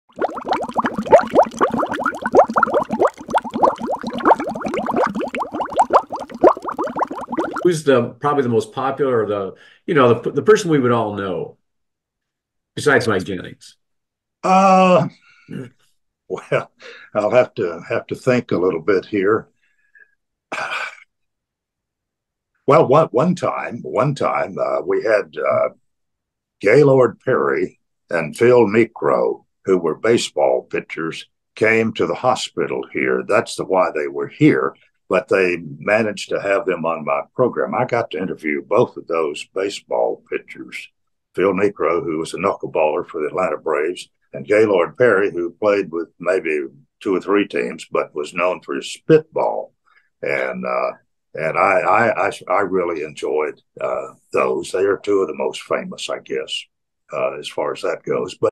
Interviewing Baseball Legends Phil Niekro and Gaylord Perry
Join me as I sit down with two iconic baseball pitchers, Phil Niekro and Gaylord Perry. Learn about their legendary careers and the secrets behind their famous pitches. Don't miss this exclusive interview!